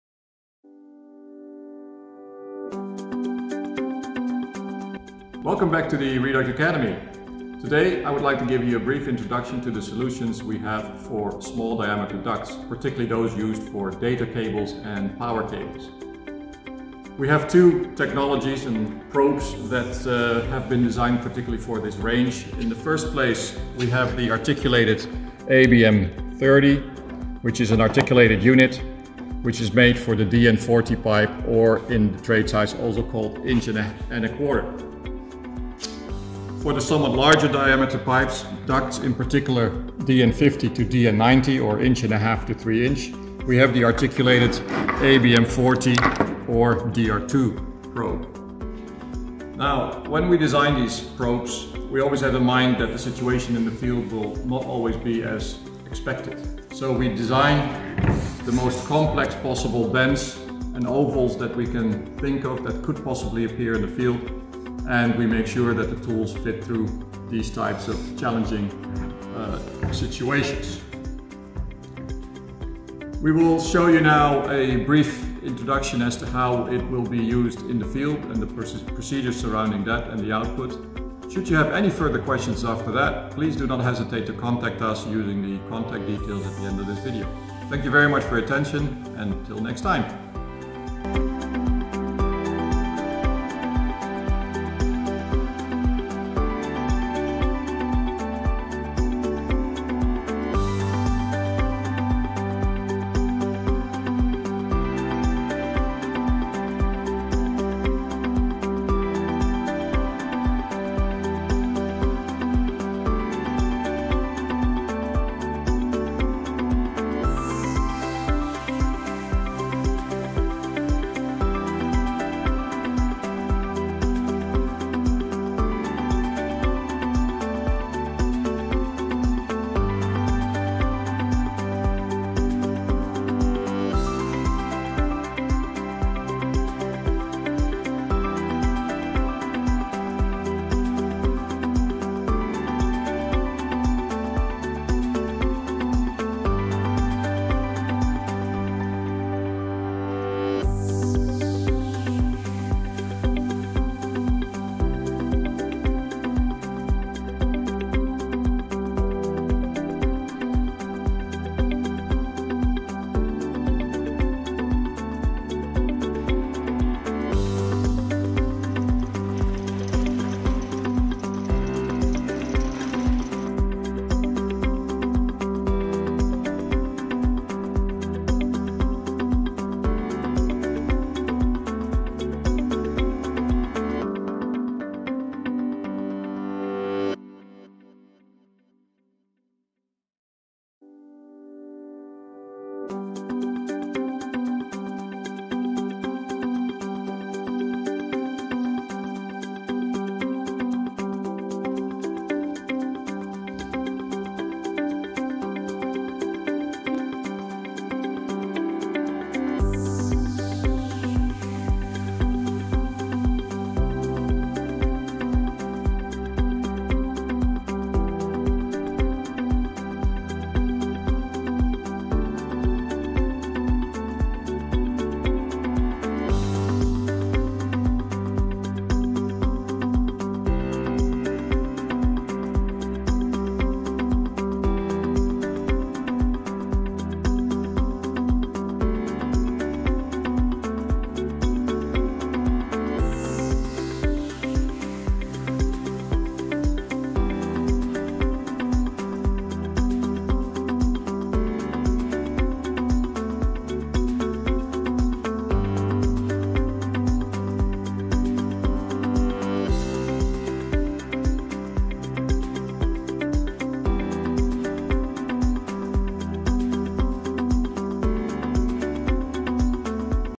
Gyro-mapping Reduct's DN50 test track with our ABM-40/ DR-2
With the arrival of 5G, creating an as-built map of your underground utilities is getting more important.

Discover how our small diameter duct mapping solutions can help improve the as-built data quality of your network.

In this video we use the ABM-40/ DR-2 gyroscopic based solutions to map Reduct's test track and we show how accurate the data is.